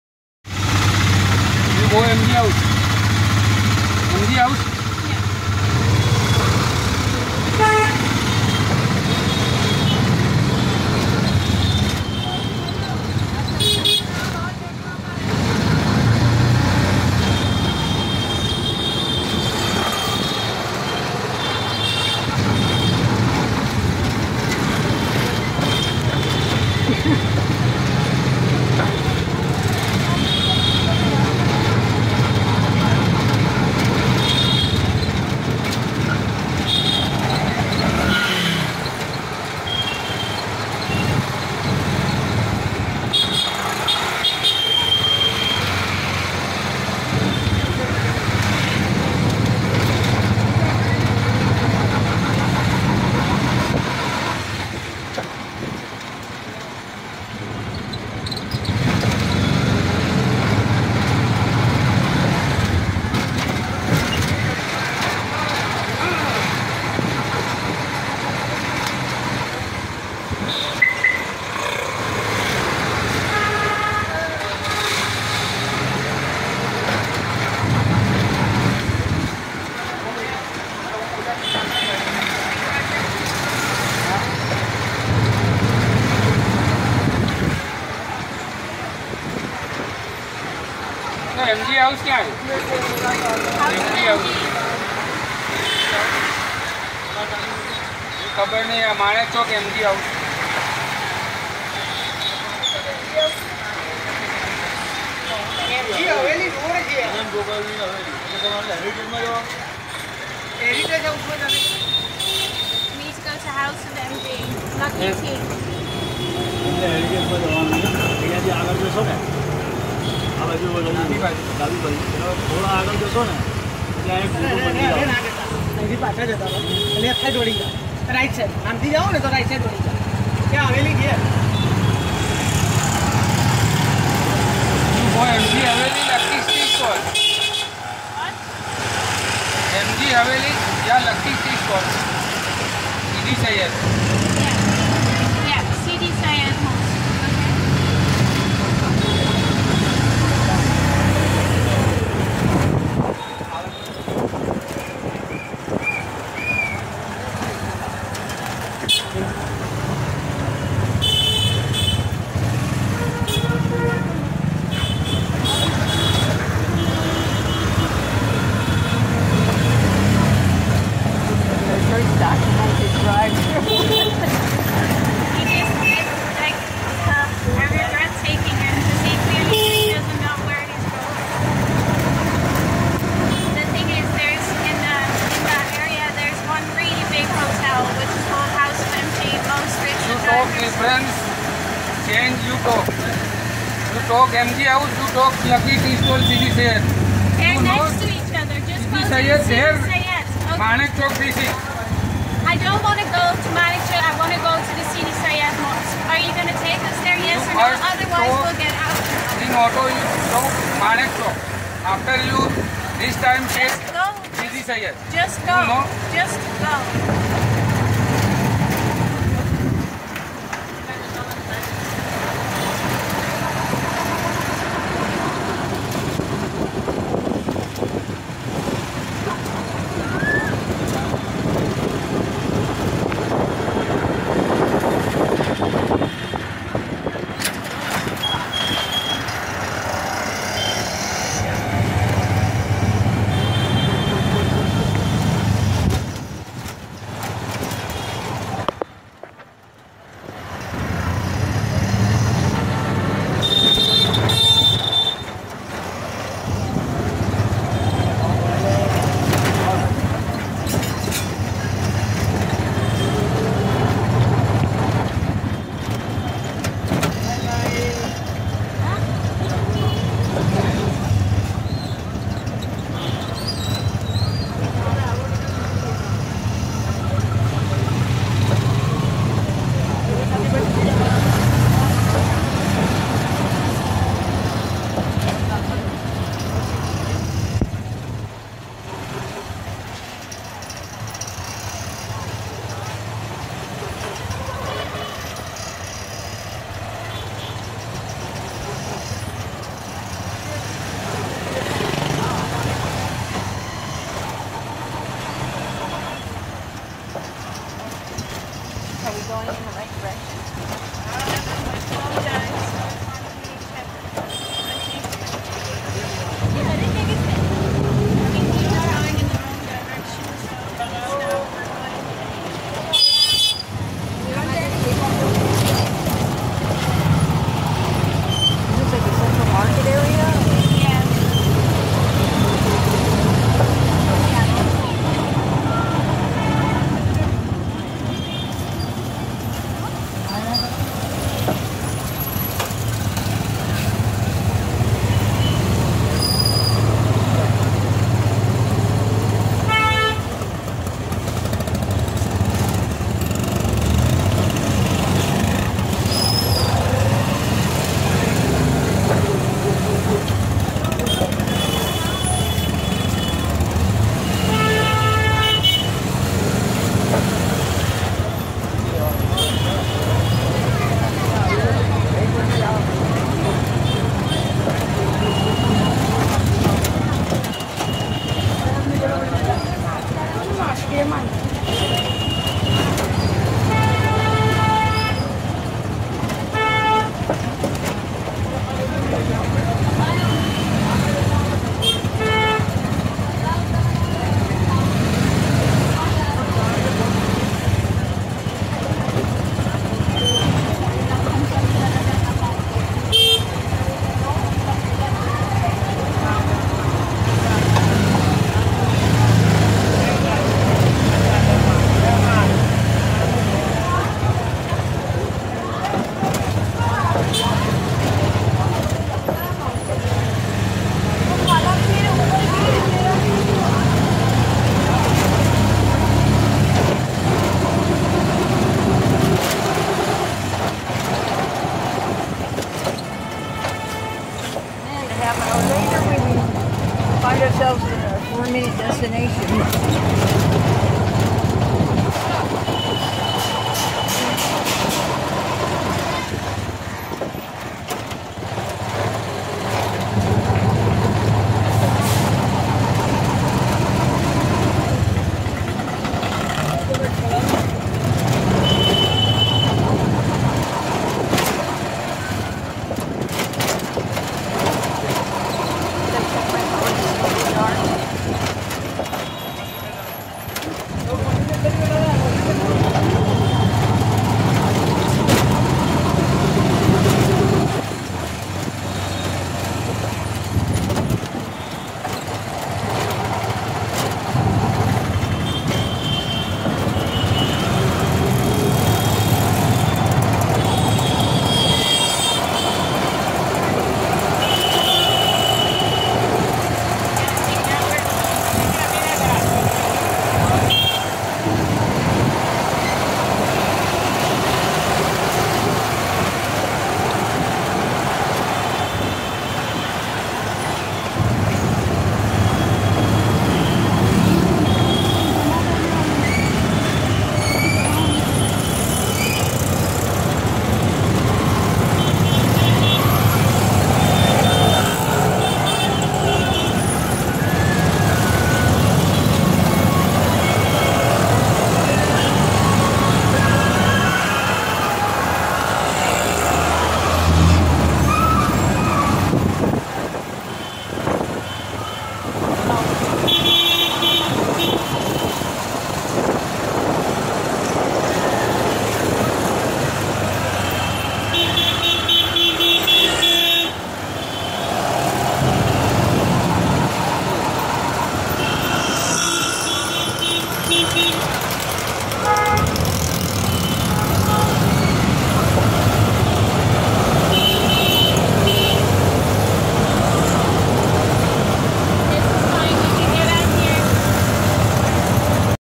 First rickshaw ride Old City.mp4
Mila negotiates our destination. Sometimes it can be like this. From Old City markets to Make our by Sidi Sayed mosque.